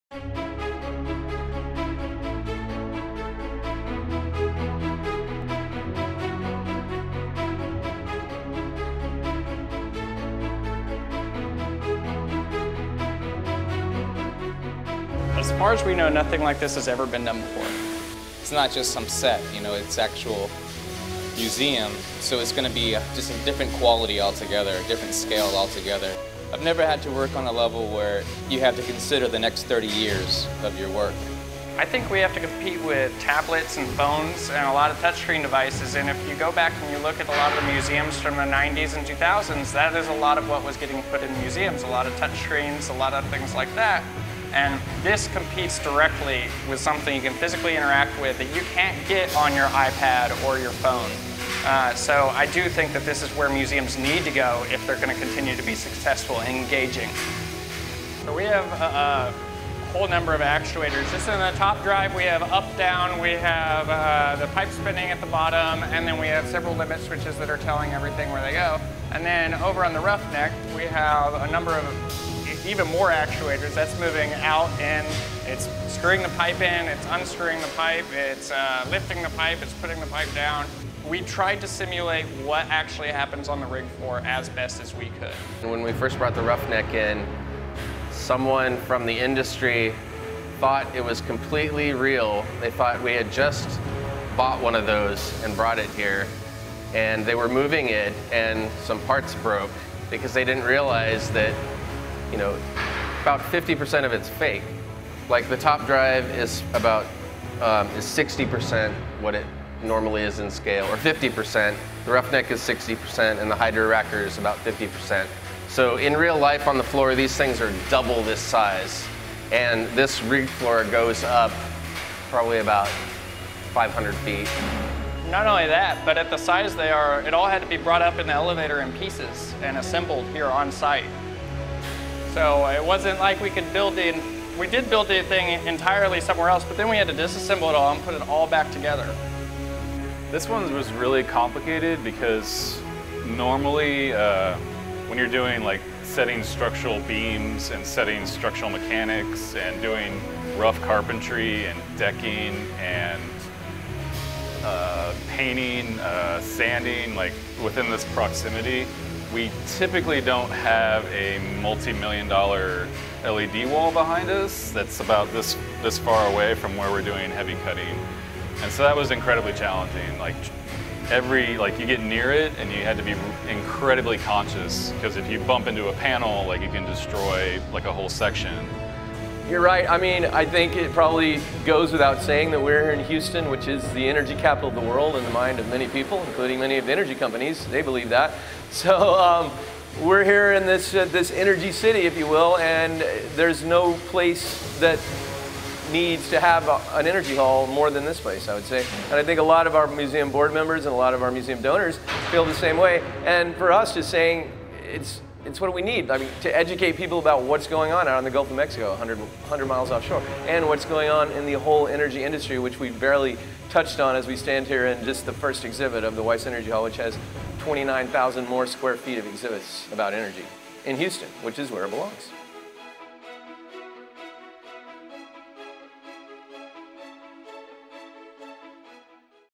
Wiess Energy Hall: Oil Rig Entry
Set by set: Our weekly tour through the new Wiess Energy Hall 3.0 begins with this awesome interview montage with some of the engineers and artists that made the spectacular gadgets and gizmos featured in the new WiessEnergyHall possible.